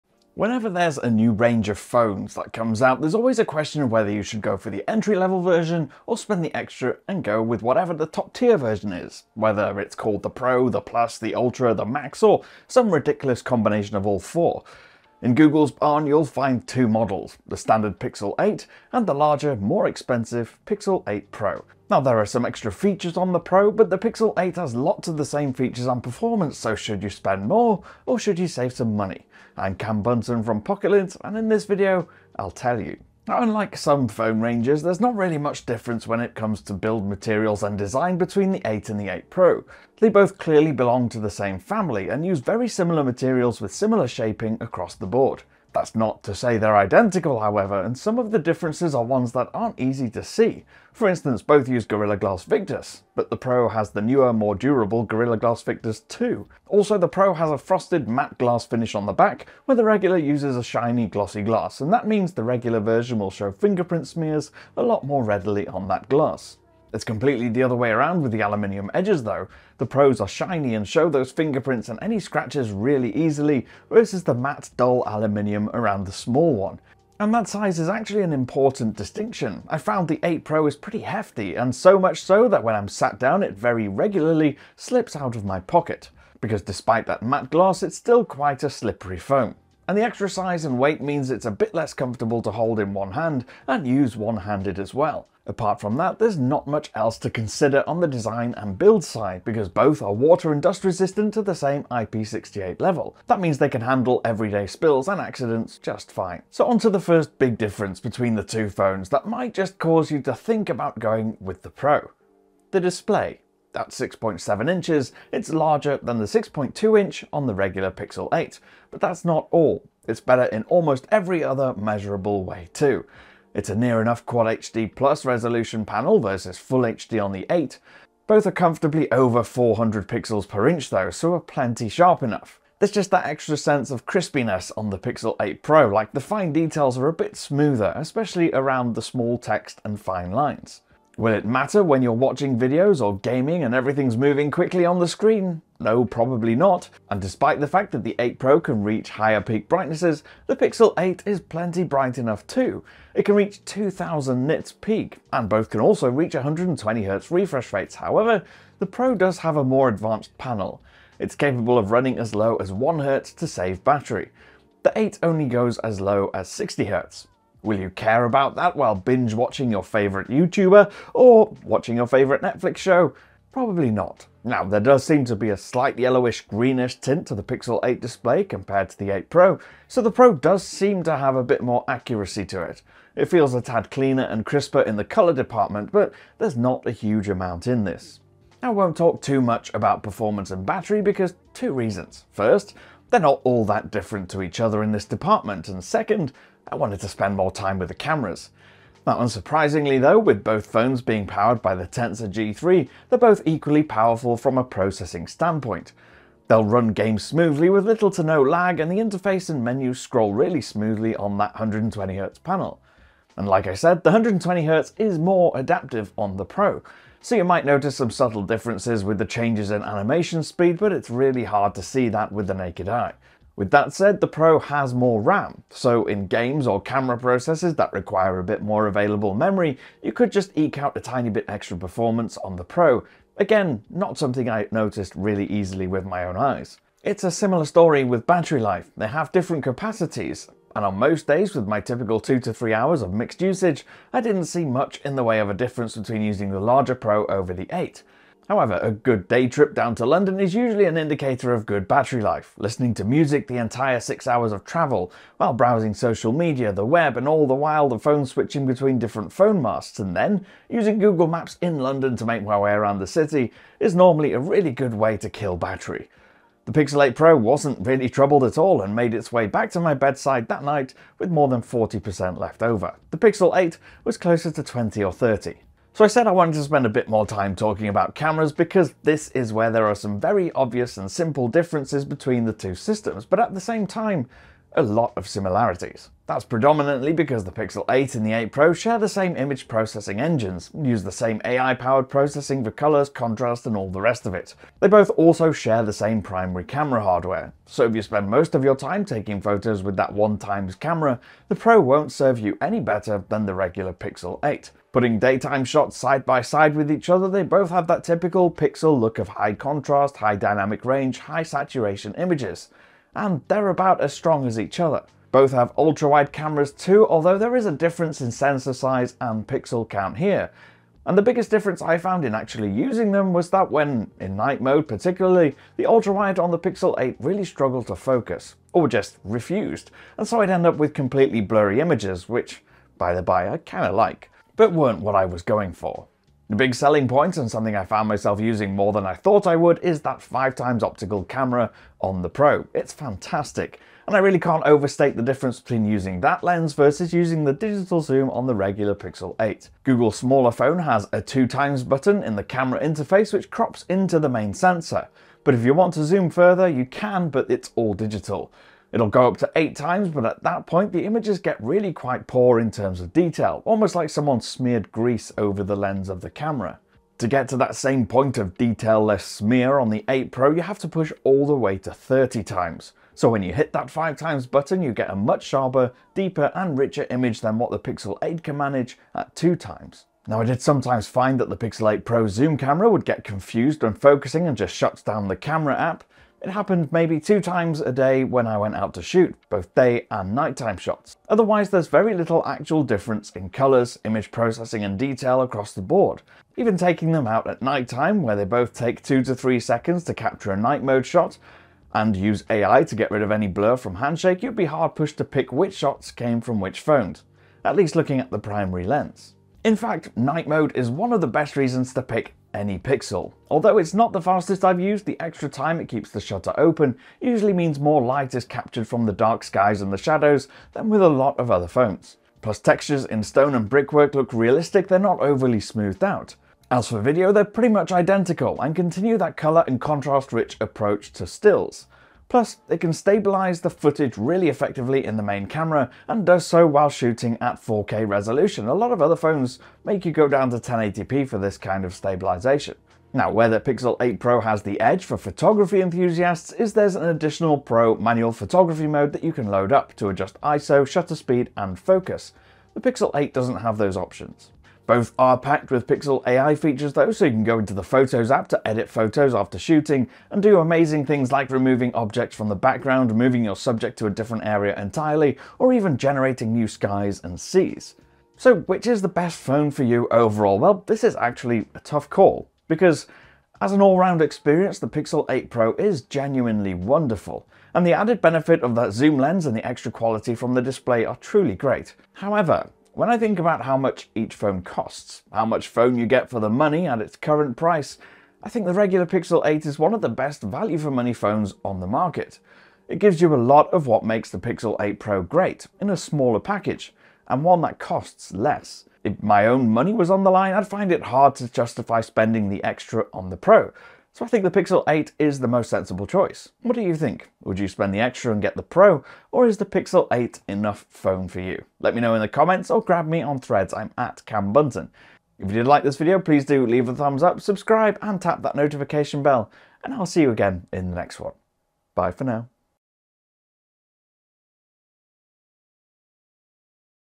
Google Pixel 8 vs Pixel 8 Pro: Should you save money?
Most companies now have a 'regular' and 'Pro' version of their flagship phones. But does it always make sense to go for the most expensive, most feature-rich model? Cam compares the Pixel 8 with the Pixel 8 Pro in this video to help you decide.

Pixel 8
Pixel 8 Pro